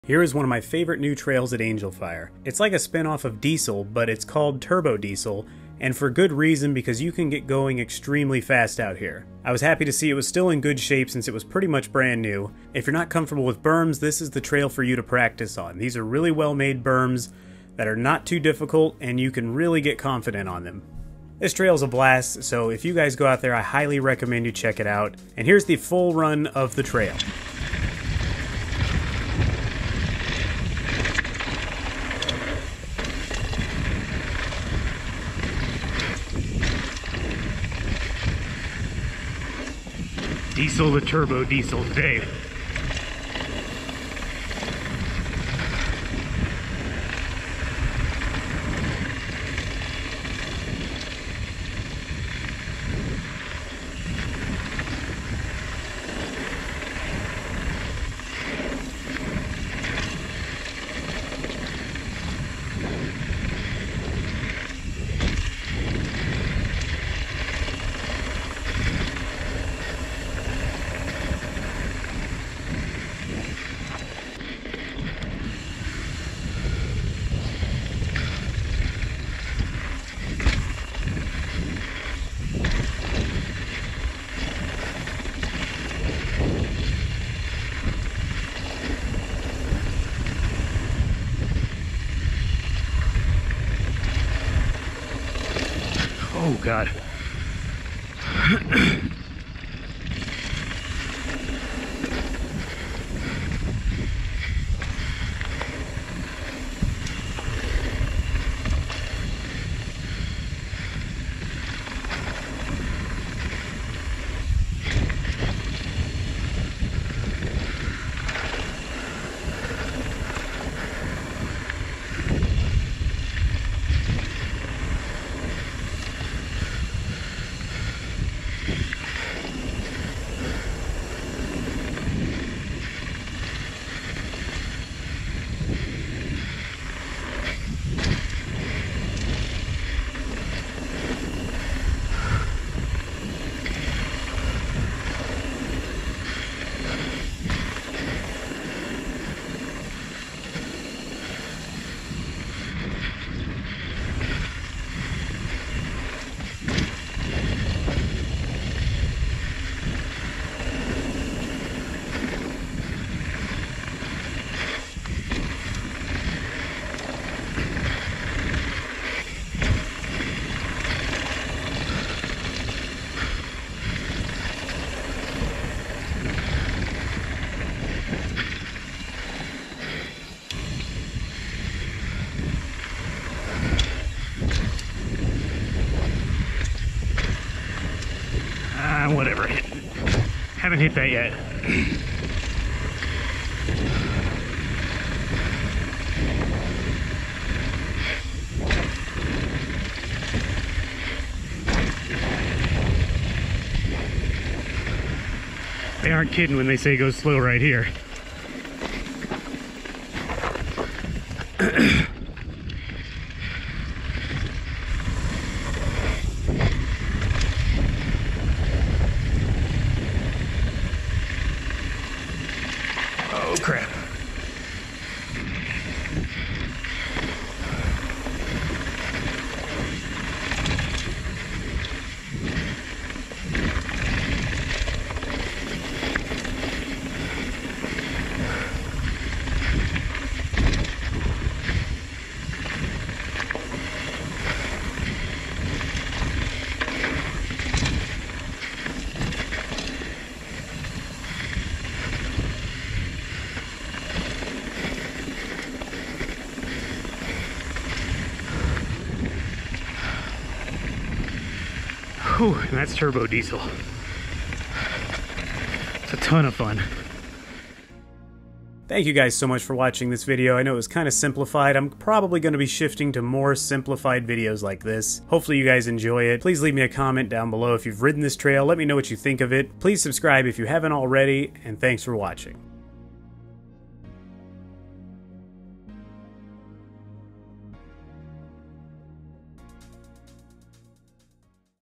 Turbo Diesel at Angel Fire Bike Park! Berms Galore!
This is one of my new favorites at Angel Fire! I highly recommend checking it out if you go. It doesn't require a lot of skill but can help build your confidence and skill level on berms.